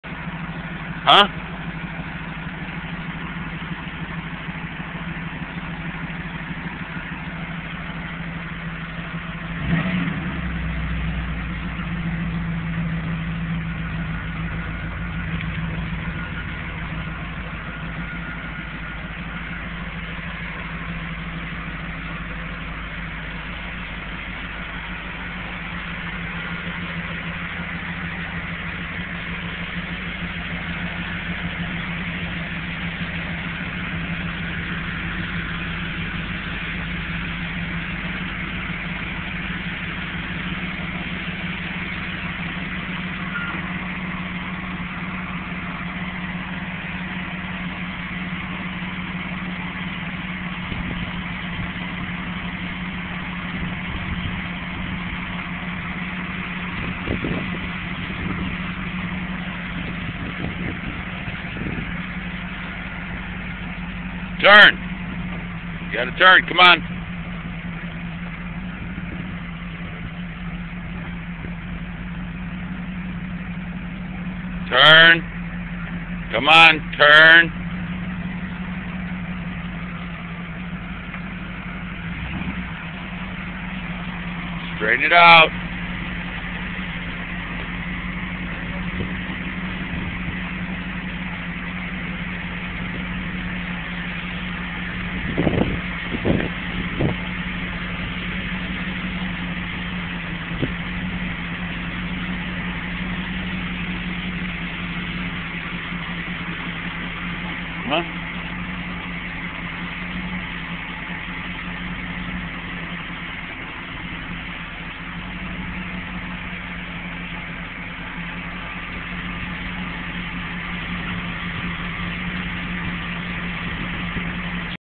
Me moving my Dad's Car
375 HORSEPOWER CAR I'M moving OMG!!!